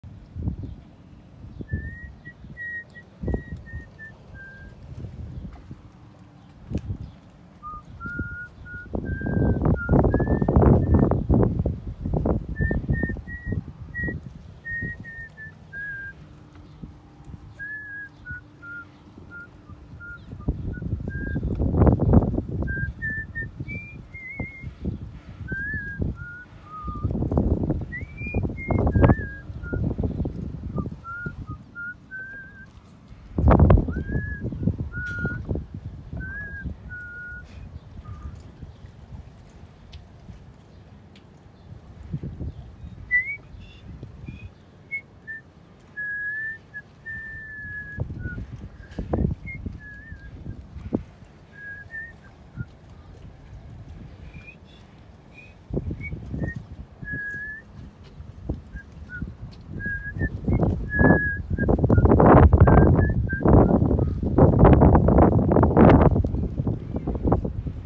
Freehand outlining a 14” tall “O” in the wind.
Martha Esch Yacht Lettering and 24 it. Gold Leaf. I also painted the Faux wood added prior to the lettering.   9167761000